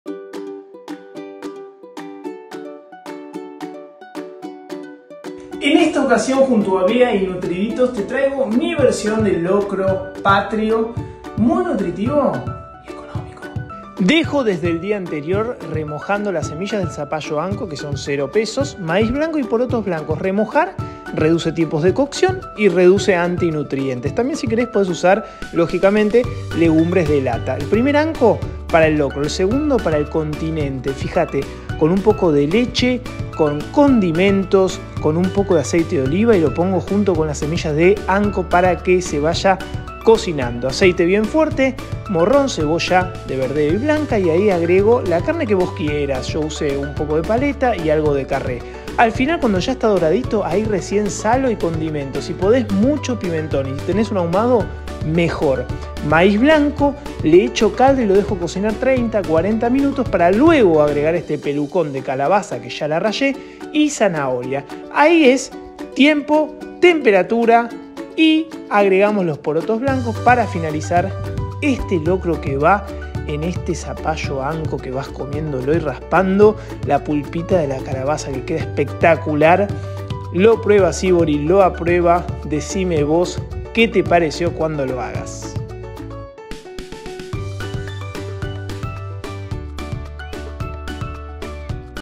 nutriditos | Locro por Diego Sivori
¡Te dejamos una super idea para festejar este 25 de mayo!🇦🇷
Seguí la receta de @sivoridiego y sorprende a todos

🍴INGREDIENTES:
1 zapallo CABUTIA rallado. Opcional tener uno mas para servir.
2 Zanahorias ralladas
1 cebolla blanca
2 cebollas de verdeo
Medio morron rojo
Ajo
200g Paleta de vaca
200g de carne de cerdo (la que prefieras)
200g de Porotos blancos
500g de Maiz blanco
Aceite de Oliva c/n
Pimienta, Ají Molido, Pimentón Dulce y Ahumado (ambos bastante), Oregano.
👨‍🍳Pasito a pasito:
1. Dejar remojando por 12 horas las semillas del zapallo y los porotos.
2. Rallar 1 zapallo y las zanahorias.
3. Dorar con aceite de oliva las cebollas (reservar parte verde para la presentación) junto con el morron, una vez dorados agregar la carne. Salar y condimentar una vez dorada la carne.
4. Agregar el maíz cubrir con caldo de verduras y cocinar por 30 minutos.
5. Agregar el zapallo y la zanahoria y cubrir conn agua. Cocinar hasta que el maíz este blando.
6. Agregar los porotos blanco cocidos y cocinar unos minutos mas.
7. Podes servir en un zapallo ancho junto con las semillas de calabaza tostadas, aceite de oliva y la parte verde de la cebolla de verdeo.

Info nutricional:
* Hidratar las legumbres y semillas reduce sus antinutrientes, lo que permite que sus nutrientes sean mejor absorbidos.
* Al reducir la cantidad de Proteína animal e incrementar la proteína vegetal, mantenemos el valor nutricional y reducimos costos.
* Este locro es reducido en grasas, un alimento super equilibrado y con buen aporte de fibra.